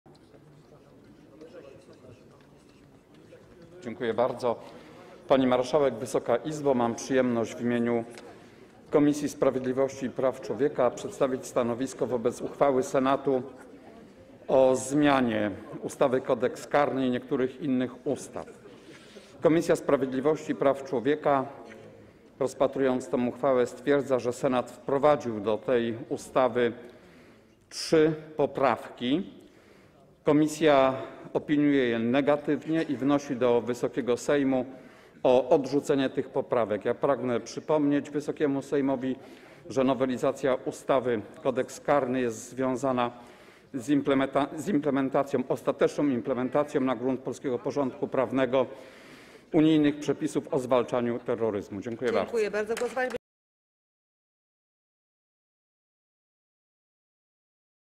Krzysztof Lipiec - wystąpienie z 20 maja 2021 r.
Popierasz? Udostępnij! Krzysztof Lipiec - wystąpienie z 20 maja 2021 r.

Sprawozdanie Komisji o uchwale Senatu w sprawie ustawy o zmianie ustawy – Kodeks karny oraz niektórych innych ustaw